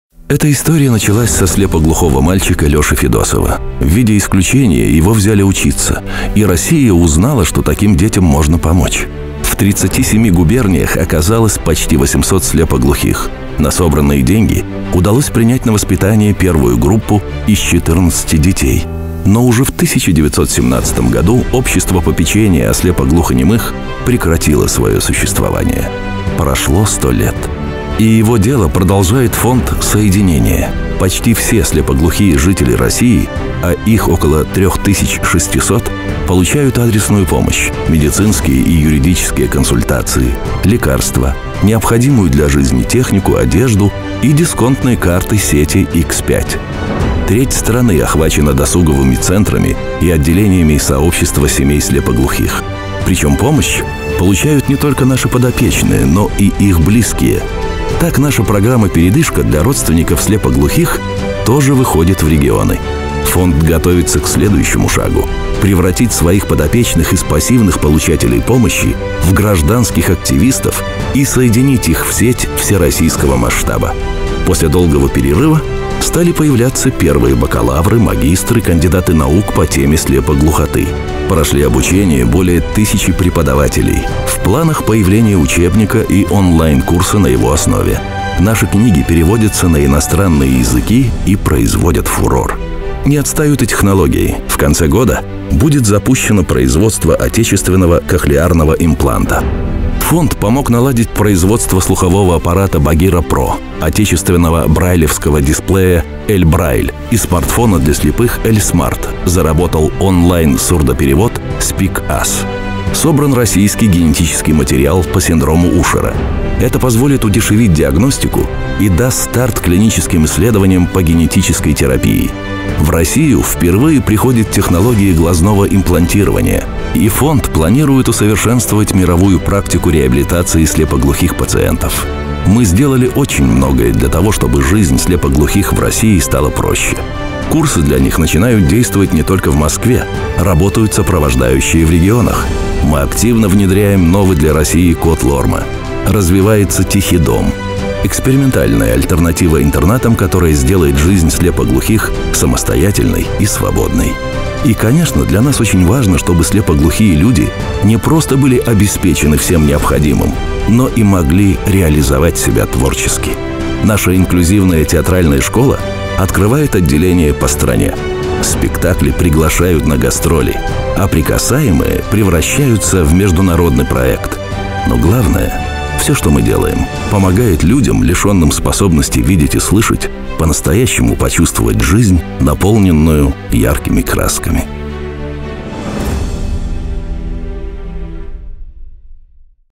Как помогают слепоглухим людям в России
Отчетный ролик о деятельности Фонда поддержки слепоглухих "Со-единение" в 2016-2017 гг.